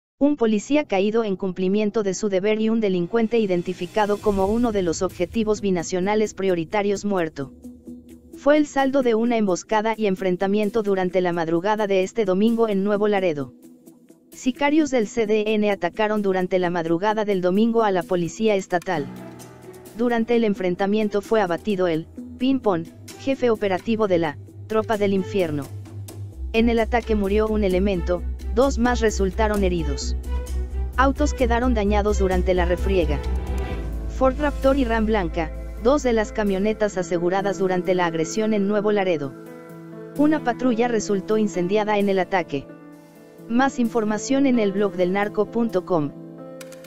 Así fue enfrentamiento entre Policías Estatale y Sicarios del Cartel del Noreste
Nuevo Laredo, Tamaulipas 29/03/20. – Un policía caído en cumplimiento de su deber y un delincuente identificado como uno de los objetivos binacionales prioritarios muerto.

Fue el saldo de una emboscada y enfrentamiento durante la madrugada de este domingo en Nuevo Laredo. Sicarios del CDN atacaron durante la madrugada del domingo a la Policía Estatal.   Durante el enfrentamiento fue abatido el «Pin-Pon», jefe operativo de la «Tropa del Infierno».